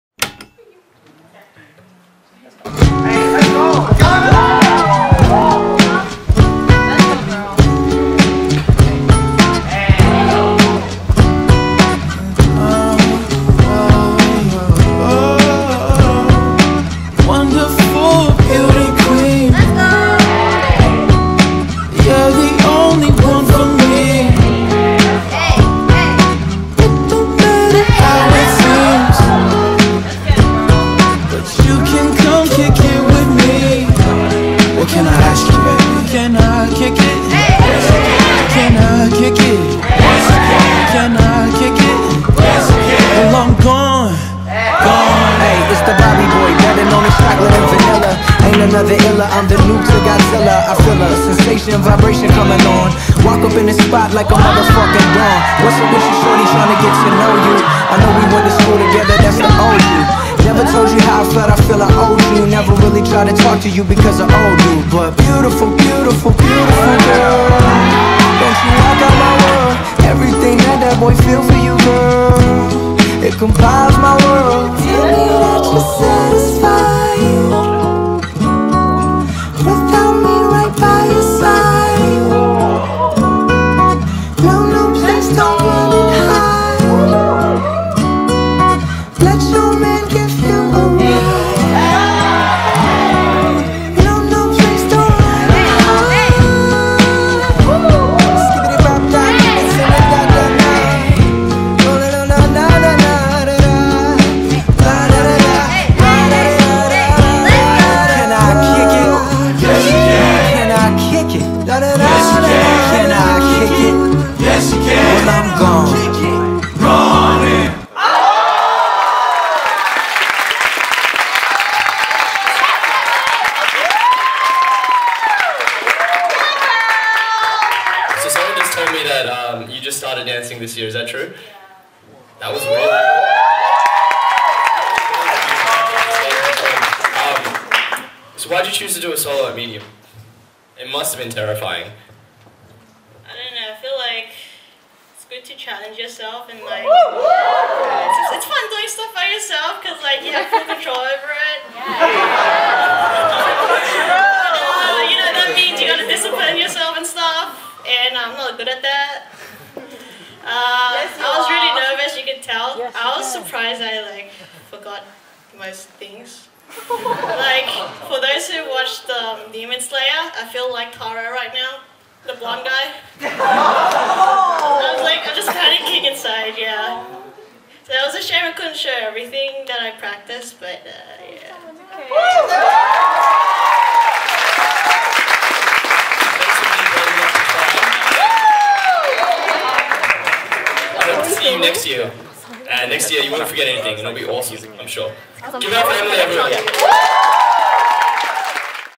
Emily Ngo | Solo Category | Medium 2019
Medium 2019: Choreography Competition

‘A platform for UNSW movers to express their movement, their stories and what dance means to them.’

Date:
Tuesday, 10th of September, 6pm - 9:30pm

Location:
Tyree Energy Technology G16
University of New South Wales (UNSW)

MC:
Robin Chen

JUDGES:
Koh Yamada
Paven Gill
Tiffany Nung